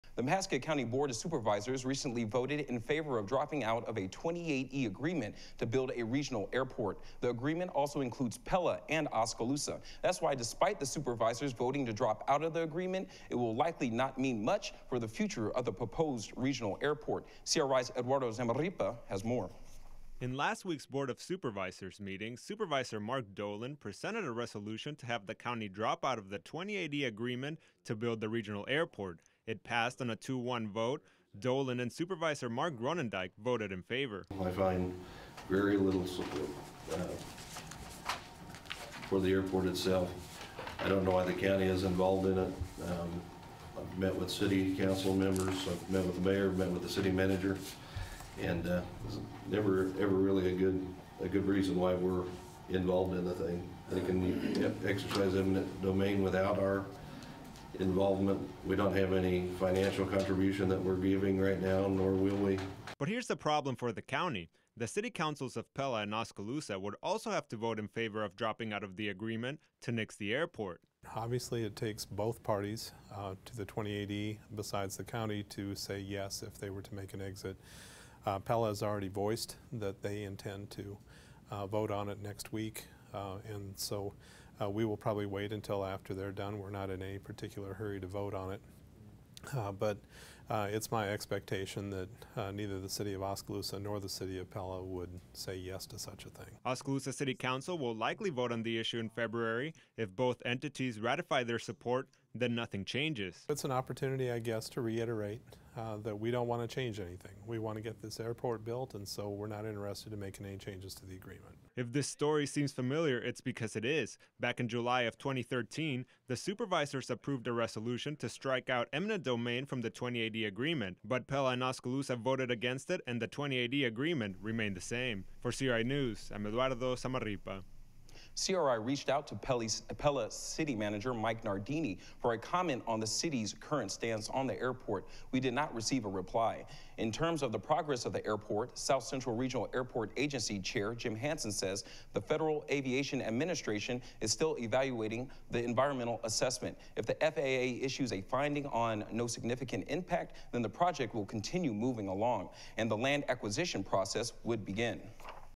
Supervisors Vote in Favor of Dropping Out of Regional Airport 28E Agreement
The Mahaska County Board of Supervisors recently voted in favor of dropping out of a 28E agreement to build a regional airport.